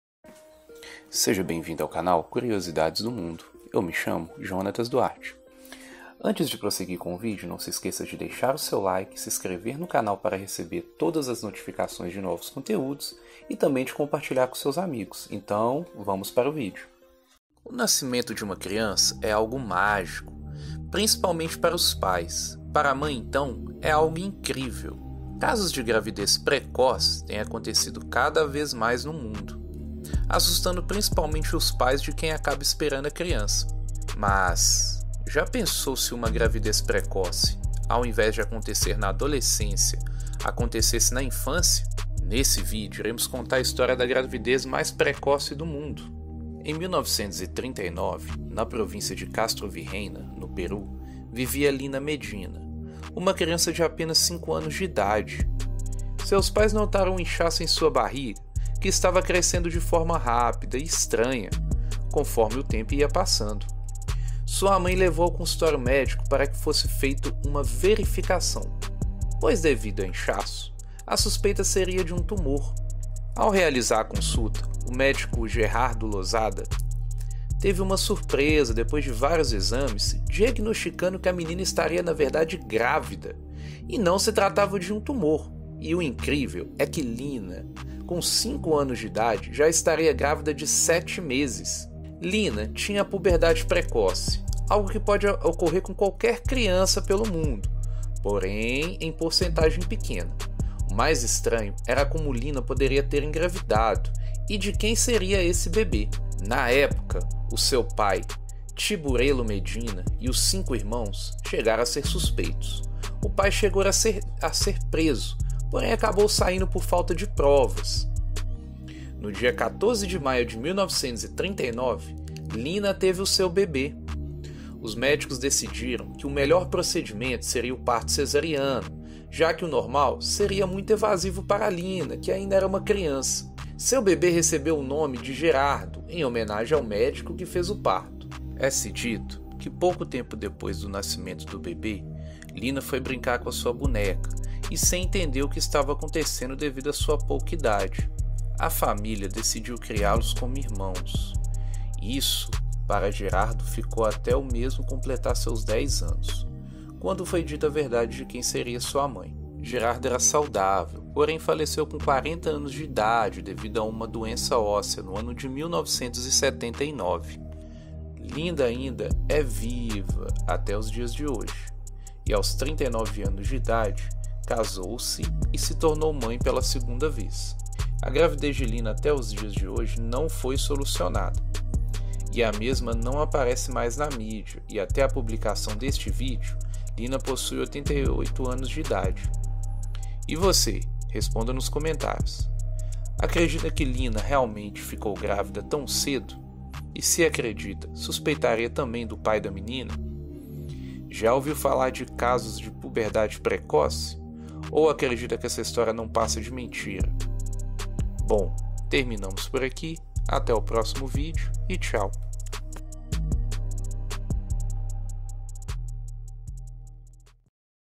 Gravida aos 5 anos de idade. Quem foi Lina Medina?
Nesse vídeo contaremos a história de Lina Medina, a gravida mais nova da história, contaremos essa história que até hoje continua um mistério
Fique a vontade para interagir.